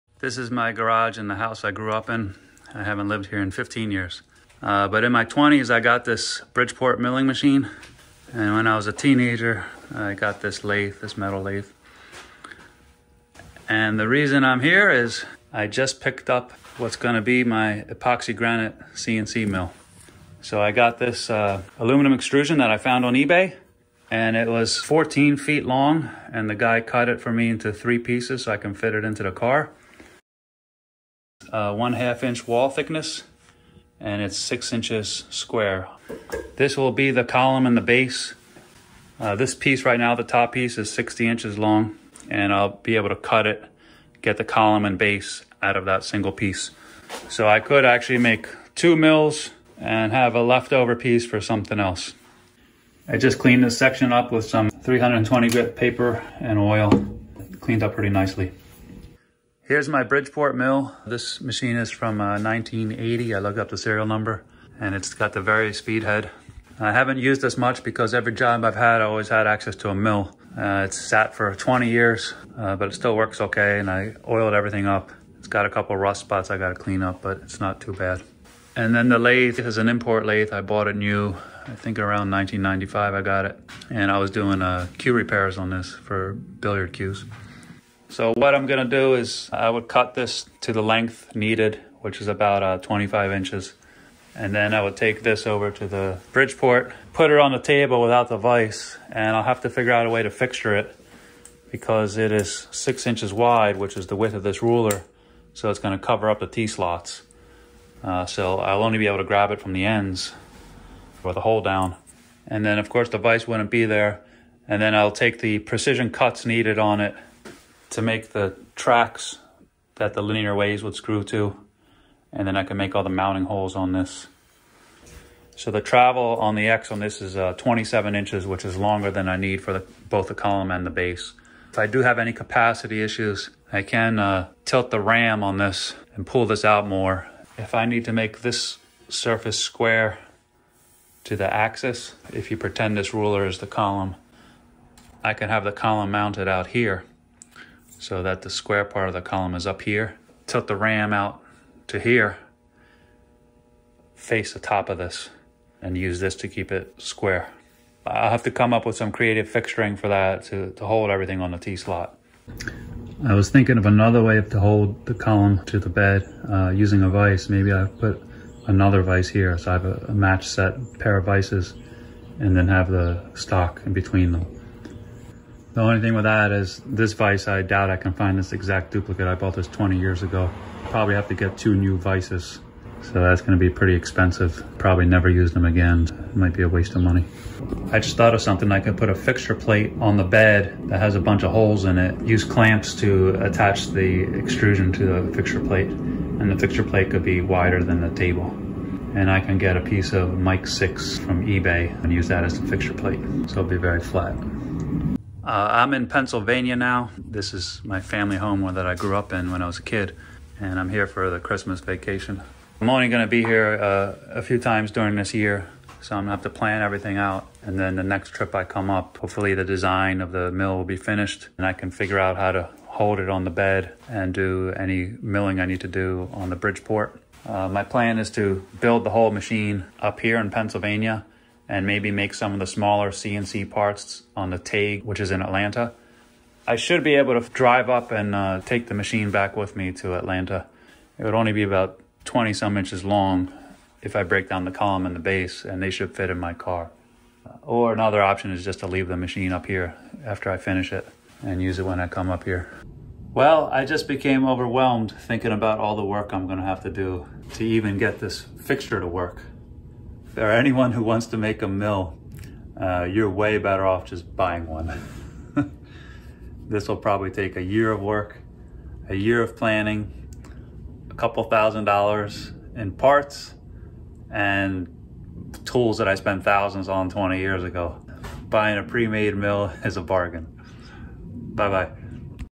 Ep11: Epoxy Granite CNC Design - Column Extrusion In-house
Ep 11: I'm in Philadelphia in my childhood home where I have my main shop. I found a 6061 aluminum extrusion locally on ebay, and bring it home. The extrusion will become both the column and the base of the CNC bench top mill. The last 30 seconds of the video are the most important! ha ha.

**Re-upload with better editing to get rid of dead air**

I'm designing an epoxy granite CNC machine, benchtop sized. The focus on this series will be on the "why" of the CNC machine design. The CAD software I'm using is Onshape. I'm a mechanical engineer, but I don't have CNC design experience, so I'll be learning as I'm going.